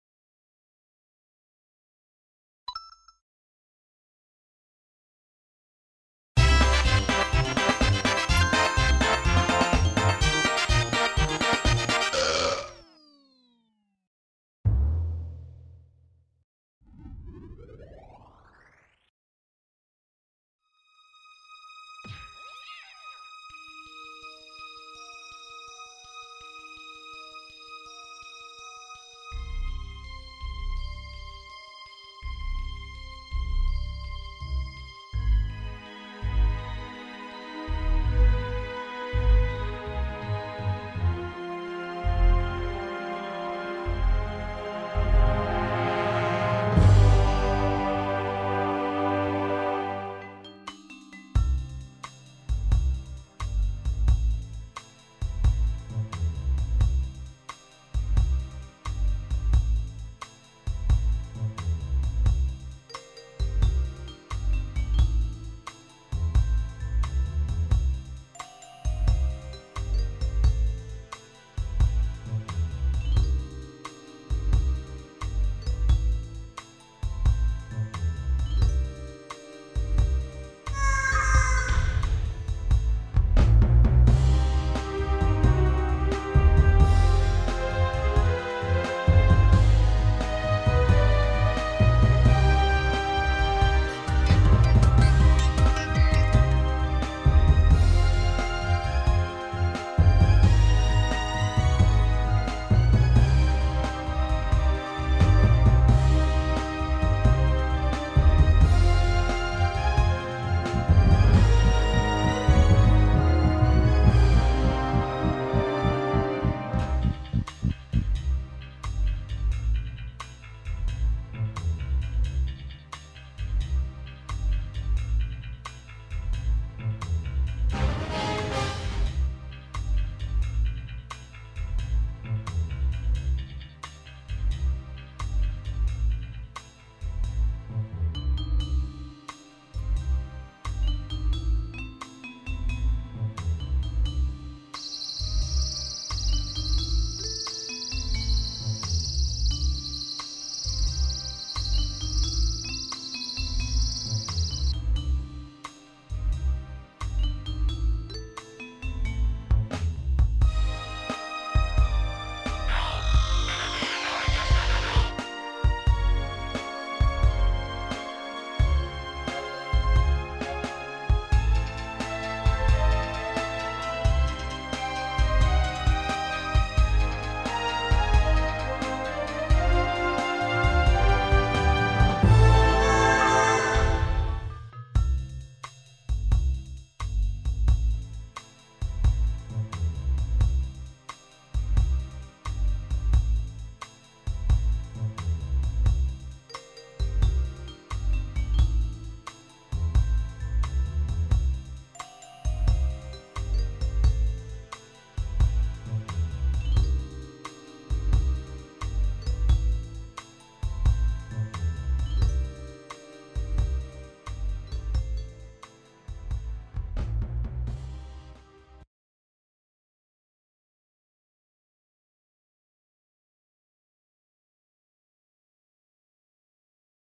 LTP3 Invitation
Orange Juice.Ribbon.Pulse.Nomad.Bomb
invite you to Lucky and Tigrou Party 3.
The coolest party of the year 1999.

X-barr                                 [code & gfx]
Traven                                [music & sfx]
 Made / BOMB!                  [add.gfx]
 Yes / Orange Juice         [motivation]